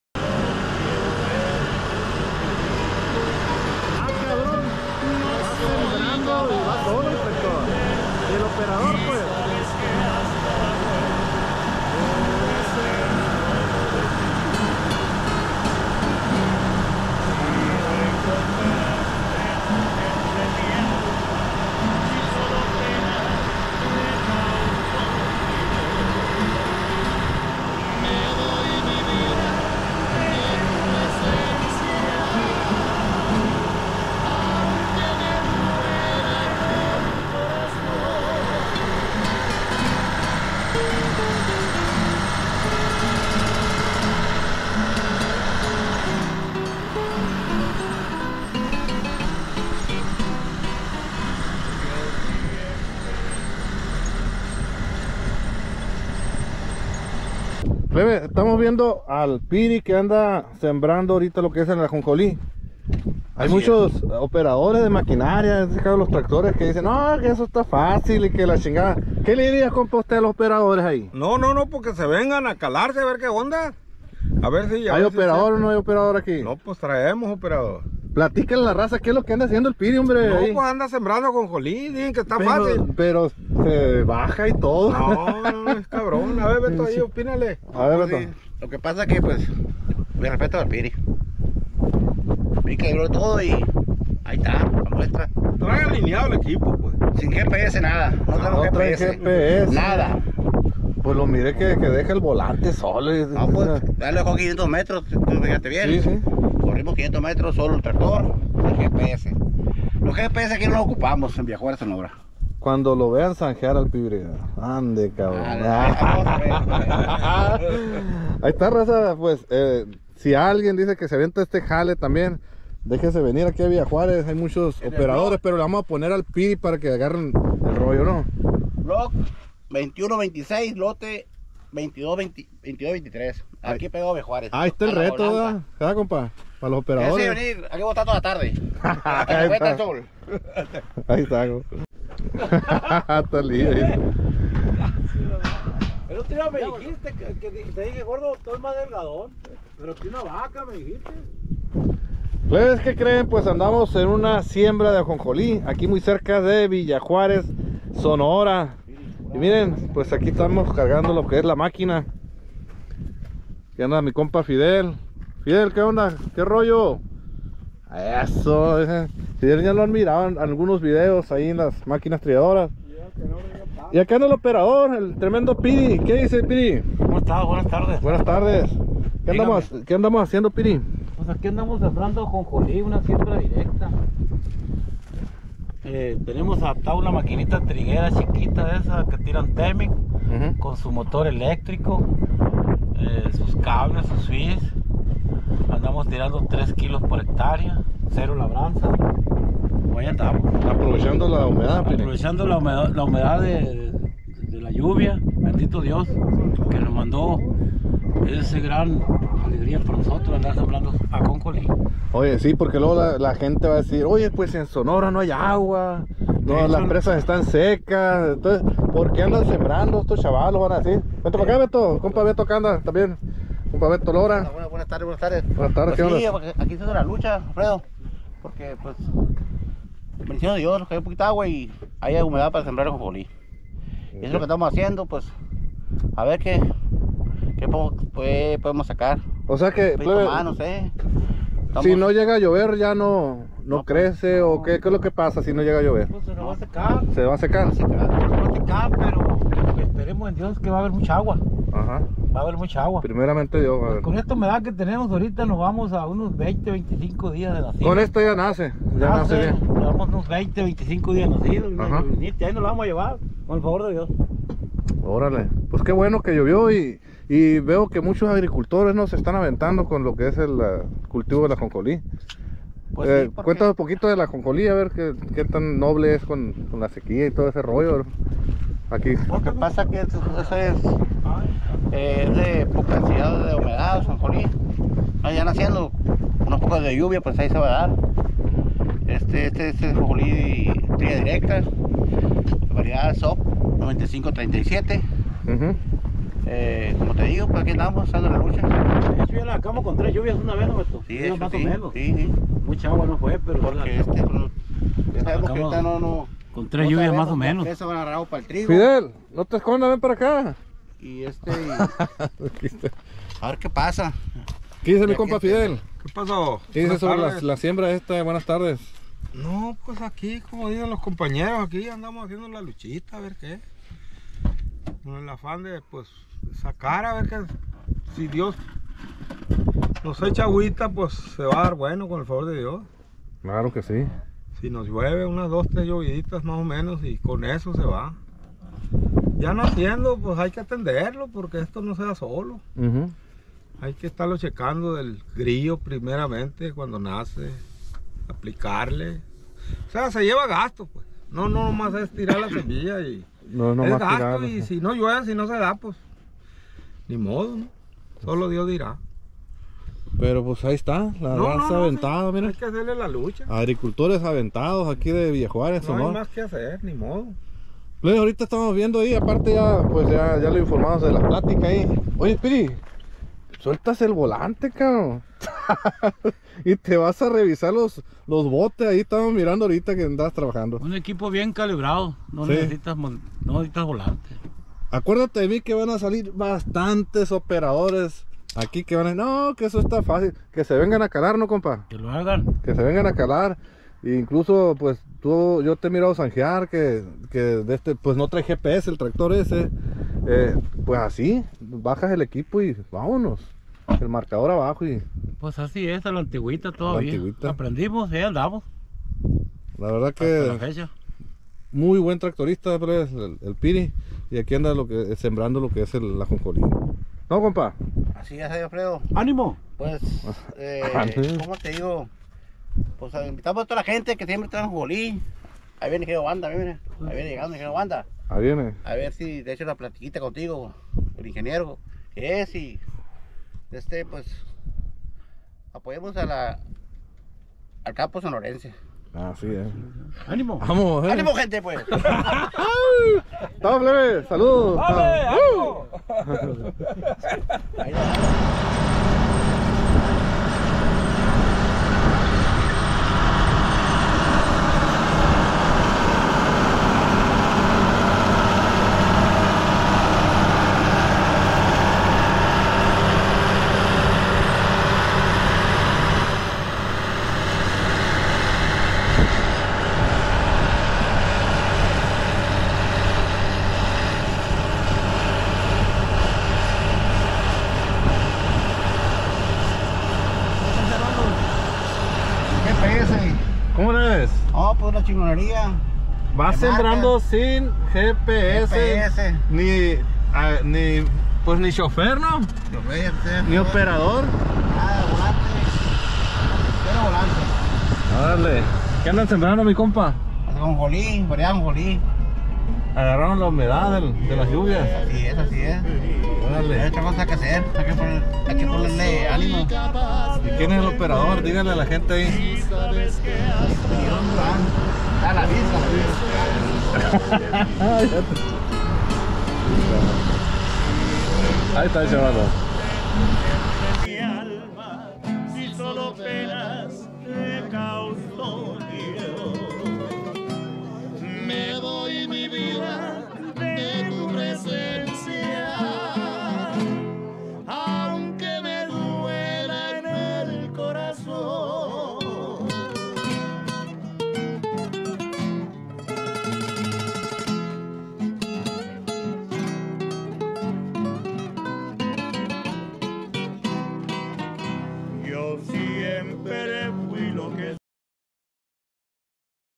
¡ INCREIBLE ! Tractor se maneja sin GPS ni operador 💚✔️Ajonjolí ☑️💚 Con la Humedad de las lluvias ✔️
¡ INCREIBLE ! El Tractor se maneja solo sin GPS Precisión en el campo💚💚✔️Siembran Ajonjolí ☑️💚 🌵 Con la Humedad de las lluvias ✔️ Villa Juárez, Sonora 💚💚viralreelsシ